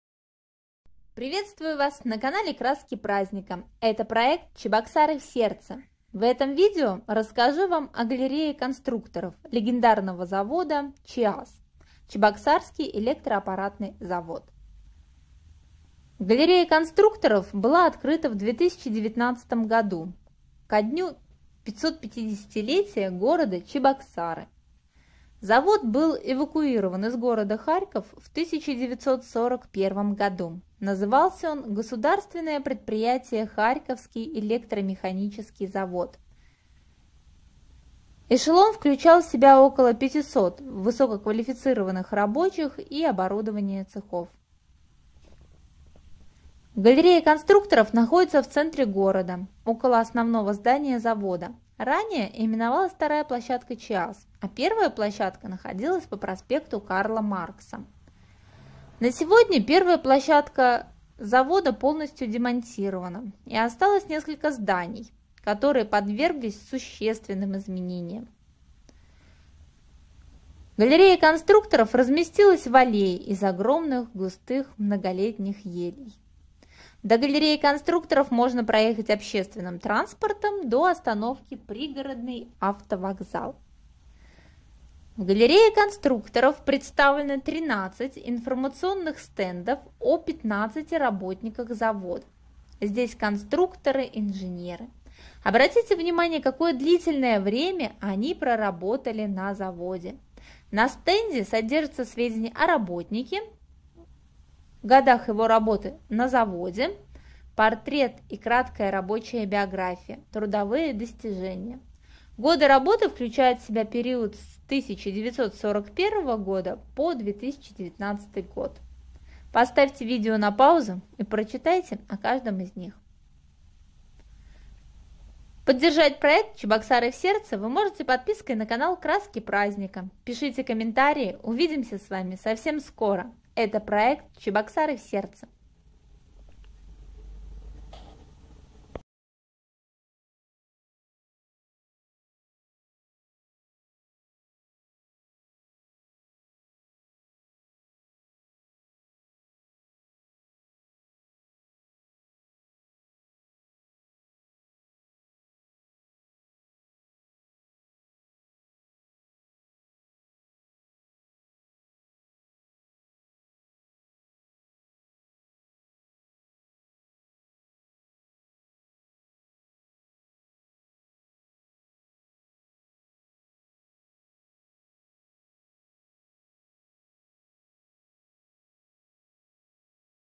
ЧЕБОКСАРЫ В СЕРДЦЕ * Галерея конструкторов ЧЭАЗ * Тринадцать стендов среди шикарных елей
Всем привет!
Это проект "Чебоксары в сердце".
В этой серии я рассказываю про Галерею конструкторов ЧЭАЗ
Дата съемки - июль 2020 года.
Рада Вашей поддержке!
Очередная серия  "Чебоксары в сердце" выходит по средам в 7:00 по Мск.